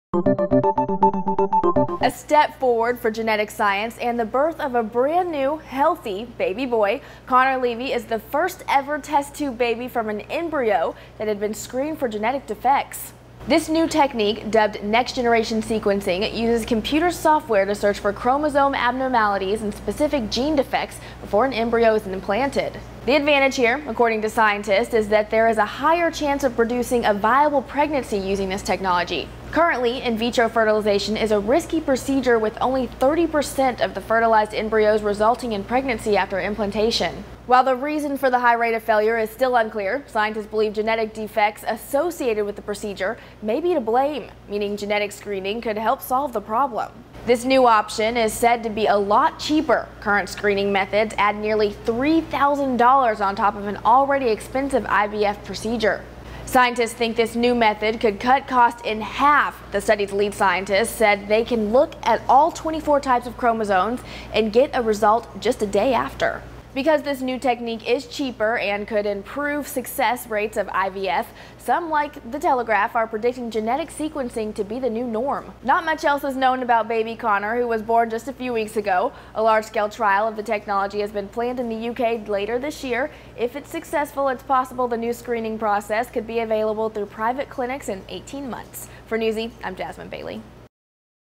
First Test-Tube Baby Born After Cheaper Genome Screening
Next-generation sequencing uses computer software to search for chromosome abnormalities and specific gene defects before an embryo is implanted.